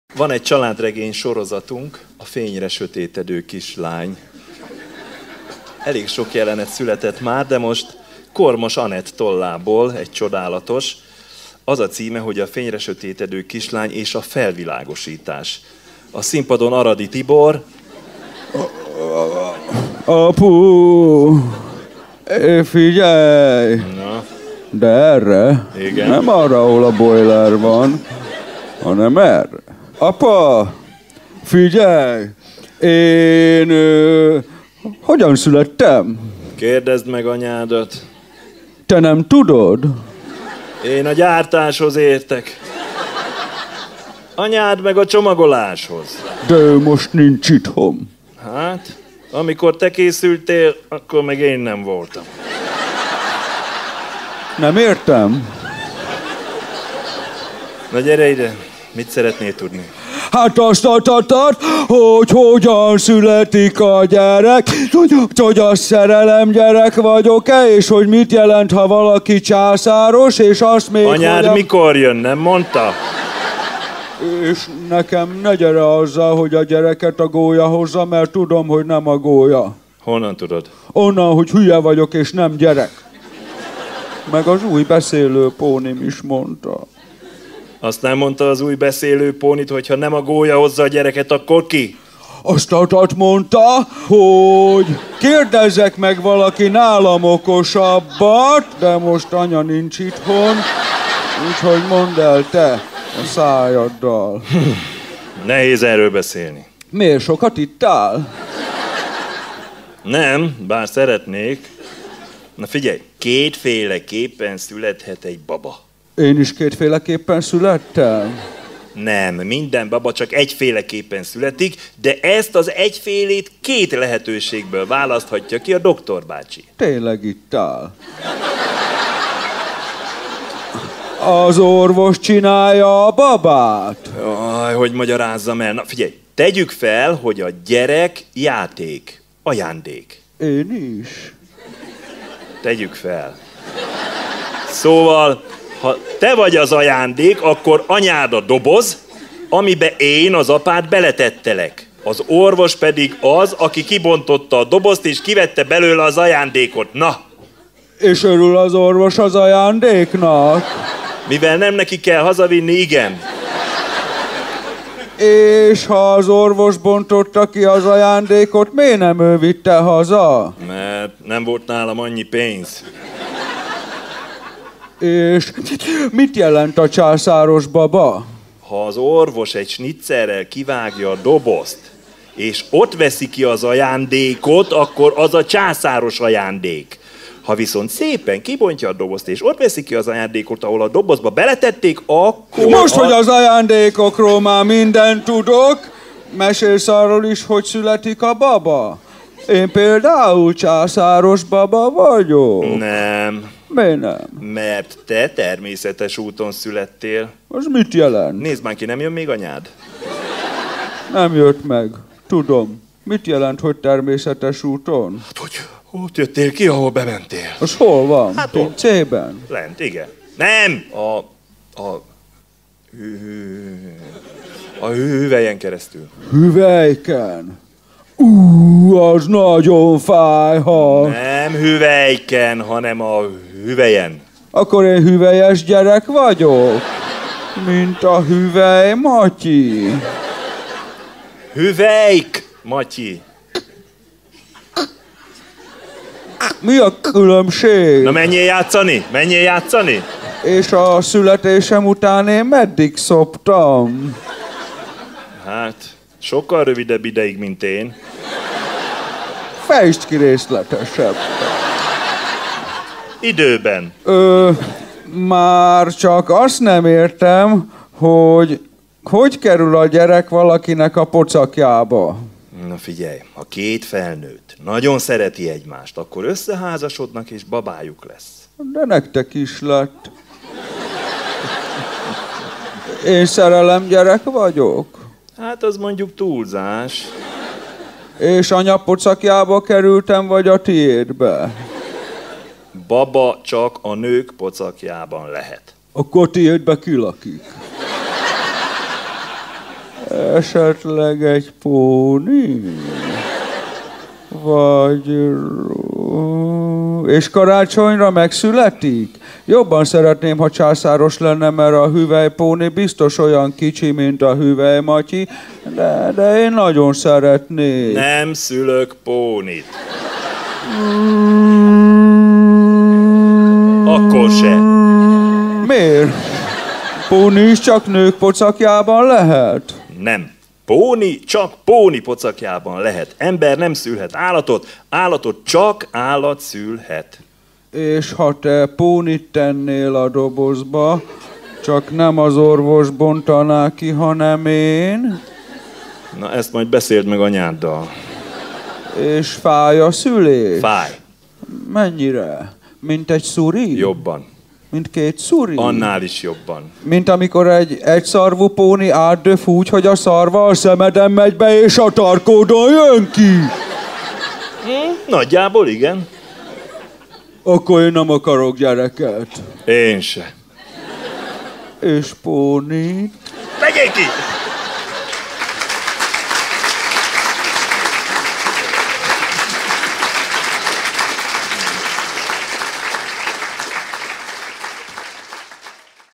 A fényre sötétedő kislány és a felvilágosítás | Dumaszínház Kabaré
A fényre sötétedő kislány visszatér! Ezúttal édesapja megpróbálja felvilágosítani.

A felvétel 2014. május 11-én készült a Corvin Dumaszínházban a Dumaszínház Kabaré Szülőnap című előadásán.